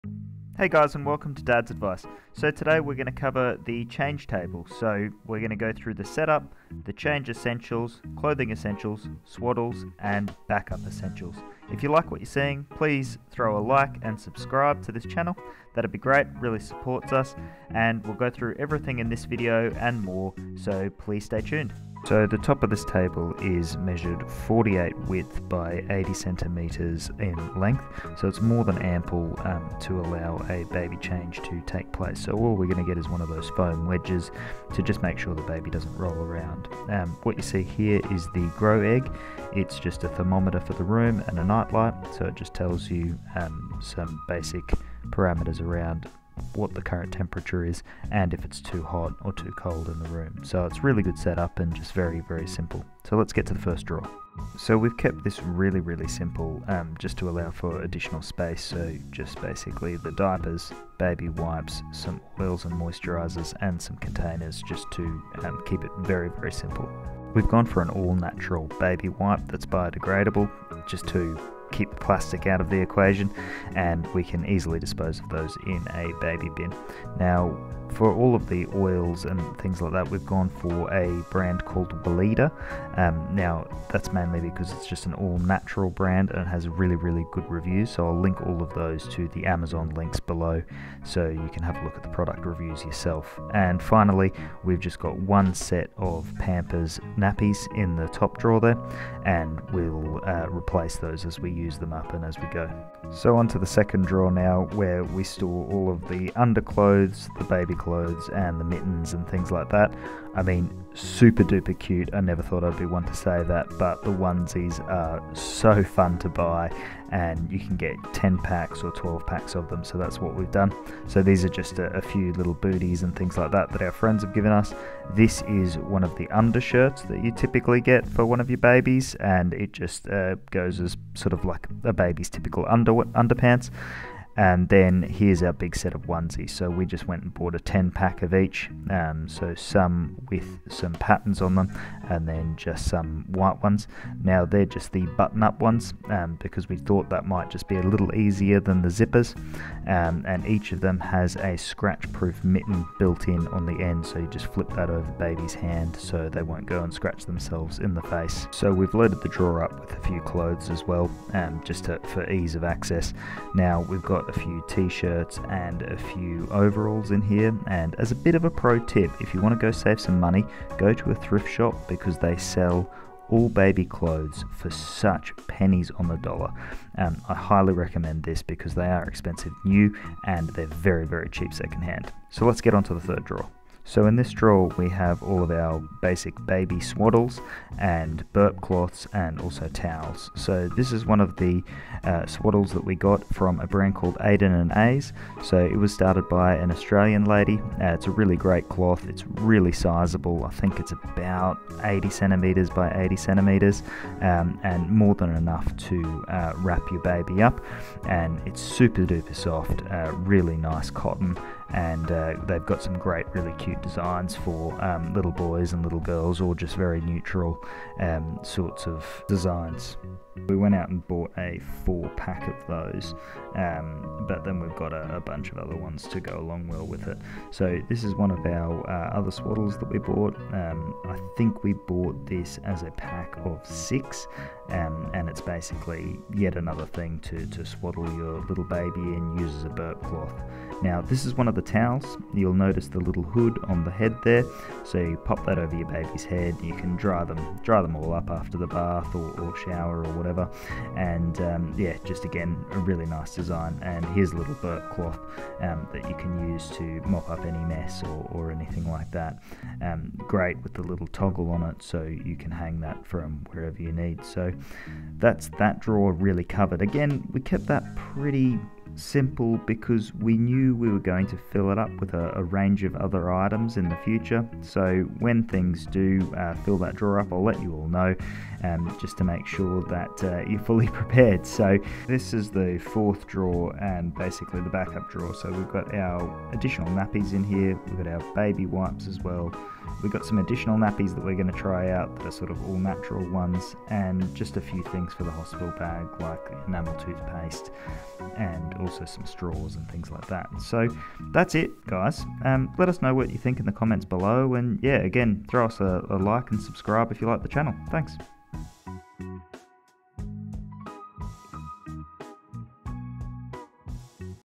Newborn Baby Change Table Essentials | First Products and Set Up
Today we look at our baby change table set up! This is our first set up and we will 100% need to buy more stuff!

And remember guys, stay relaxed, enjoy the journey and always use your products as directed ;)